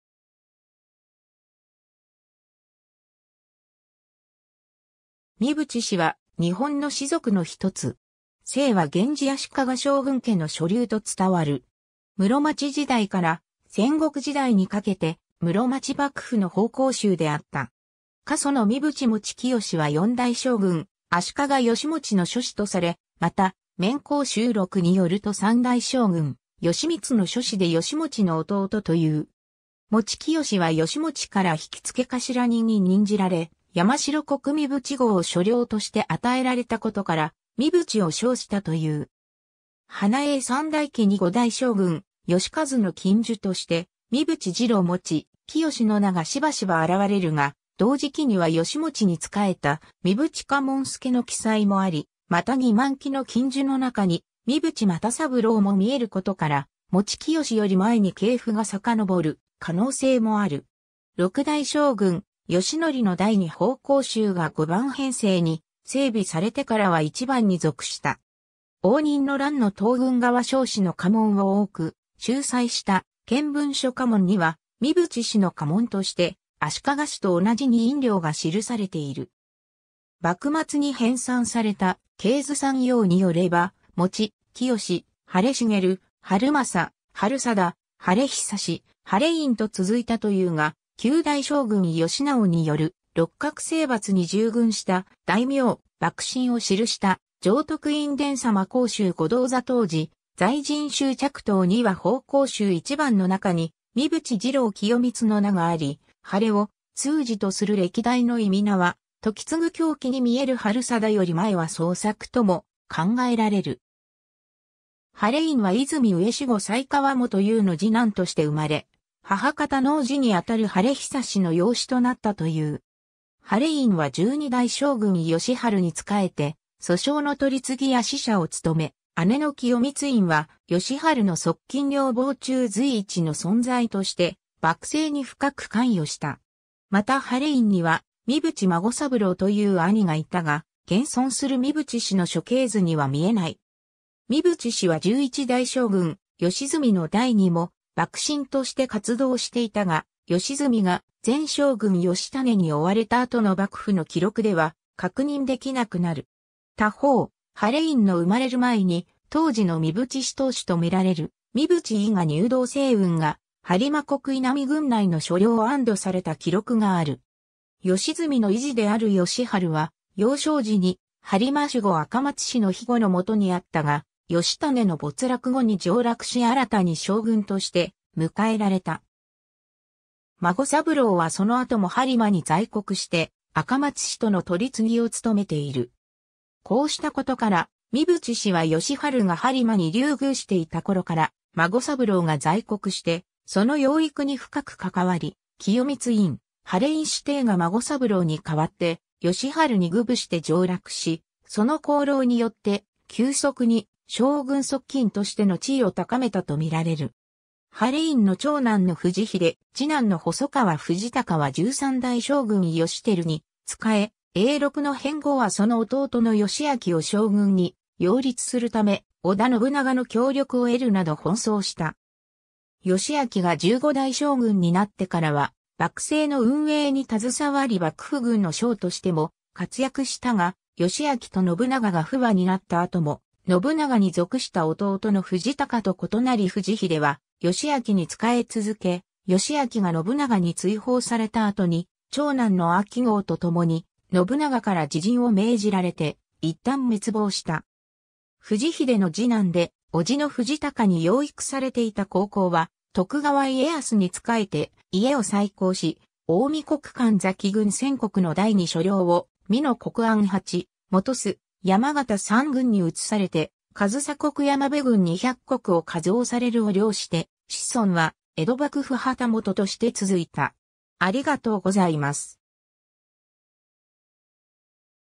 三淵氏
三淵氏（みつぶちし）は、日本の氏族の一つ。
清和源氏（河内源氏）足利将軍家の庶流と伝わる。
室町時代から戦国時代にかけて室町幕府の奉公衆であった。
 家祖の三淵持清は4代将軍足利義持の庶子とされ、また『綿考輯録』によると3代将軍義満の庶子（生母は細川氏）で義持の弟という。
持清は義持から引付頭人に任じられ、山城国三淵郷（三淵庄、三淵邑とも）を所領として与えられたことから三淵を称したという。
『花営三代記』に5代将軍義量の近習として「三淵次郎持清」の名がしばしば現れるが、同時期には義持に仕えた「三淵掃部助」の記載もあり、また義満期の近習の中に「三淵又三郎」も見えることから、持清より前に系譜が遡る可能性もある。
 6代将軍義教の代に奉公衆が五番編成に整備されてからは一番に属した。
応仁の乱の東軍側将士の家紋を多く収載した『見聞諸家紋』には、三淵氏の家紋として足利氏と同じ二引両が記されている。
幕末に編纂された『系図纂要』によれば「持清―晴重―晴政―晴貞―晴恒―晴員」と続いたというが、9代将軍義尚による六角征伐に従軍した大名・幕臣を記した『常徳院殿様江州御動座当時在陣衆着到』には奉公衆一番の中に「三淵次郎清光」の名があり、「晴」を通字とする歴代の諱は『言継卿記』に見える晴貞より前は創作とも考えられる。
 晴員は和泉上守護細川元有の次男として生まれ、母方の伯父にあたる晴恒の養子となったという。
晴員は12代将軍義晴に仕えて訴訟の取次や使者を務め、姉の清光院（佐子局）は義晴の側近女房中随一の存在として幕政に深く関与した。
また晴員には「三淵孫三郎」という兄がいたが、現存する三淵氏の諸系図には見えない。
三淵氏は11代将軍義澄の代にも幕臣として活動していたが、義澄が前将軍義稙に逐われた後の幕府の記録では確認できなくなる。
他方、晴員の生まれる前に当時の三淵氏当主とみられる「三淵伊賀入道正運」が播磨国印南郡内の所領を安堵された記録がある。
義澄の遺児である義晴は幼少時に播磨守護赤松氏の庇護の下にあったが、義稙の没落後に上洛し新たに将軍として迎えられた。
孫三郎はその後も播磨に在国して赤松氏との取次を務めている。
こうしたことから、三淵氏は義晴が播磨に流遇していた頃から孫三郎が在国してその養育に深く関わり、清光院・晴員姉弟が孫三郎に代わって義晴に供奉して上洛し、その功労によって急速に将軍側近としての地位を高めたと見られる。
 晴員の長男の藤英・次男の細川藤孝は13代将軍義輝に仕え、永禄の変後はその弟の義昭を将軍に擁立するため織田信長の協力を得るなど奔走した。
義昭が15代将軍になってからは幕政の運営に携わり幕府軍の将としても活躍したが、義昭と信長が不和になった後も、信長に属した弟の藤孝と異なり藤英は義昭に仕え続け、義昭が信長に追放された後に長男の秋豪と共に信長から自刃を命じられていったん滅亡した。
 藤英の次男で、叔父の藤孝に養育されていた光行は徳川家康に仕えて家を再興し、近江国神崎郡1,000石（子の藤利（尚正）の代に所領を美濃国安八・本巣・山県三郡に移されて上総国山辺郡200石を加増される）を領して子孫は江戸幕府旗本として続いた。